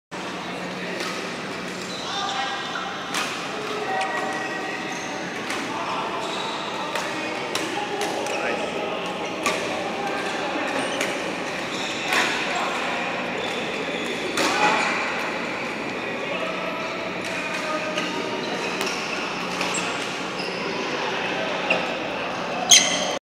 2013-12-03 21:50MYT at the Sports Arena Bukit Serd
There are 13 type of sports at the Sports Arena Bukit Serdang;  Badminton,  Sepak Takraw,  Handball,  Martial Arts,  Dance,  Snooker / Pool,  Futsal, Netball,  Dodgeball,  Yoga, Gym, Dart, and Body Bu